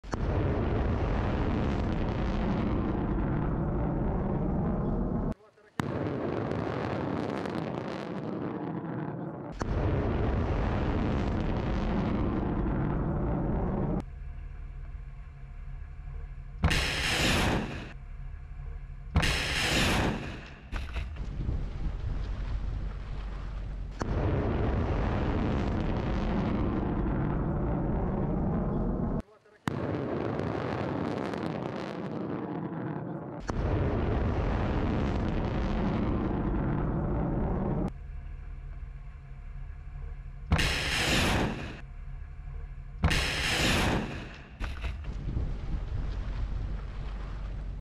Viral : रूसी नौसेना ने जापान के सागर में मॉक टारगेट पर एंटी-शिप मिसाइल दाग़ी
रूसी रक्षा मंत्रालय ने मंगलवार तड़के टेलीग्राम पर कहा, "जापान सागर के पानी में, प्रशांत बेड़े की मिसाइल नौकाओं ने मॉस्किट क्रूज मिसाइलों को दुश्मन के समुद्री लक्ष्य पर दागा।" दो जहाजों ने अभ्यास में भाग लिया था।
"लगभग 100 किलोमीटर की दूरी पर स्थित लक्ष्य को दो मोस्किट क्रूज मिसाइलों के सीधे प्रहार से सफलतापूर्वक निशाना बनाया गया।"

"In the waters of the Sea of Japan, missile boats of the Pacific Fleet fired Moskit cruise missiles at a mock enemy sea target," the Russian Defence Ministry said on Telegram early on Tuesday, adding that two ships had taken part in the exercise.
"The target, located at a distance of about 100 kilometers, was successfully targeted by a direct hit from two Moskit cruise missiles."